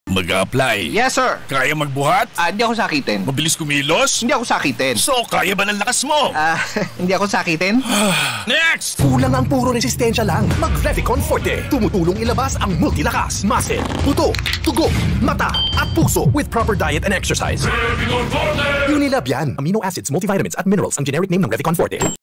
Revicon Forte Radio Commercial 2022 22s
A Radio Commercial for Revicon Forte this 2022. Unilab 'Yan!

DRPS Two 1418: Revicon Forte Radio Commercial 2022 22s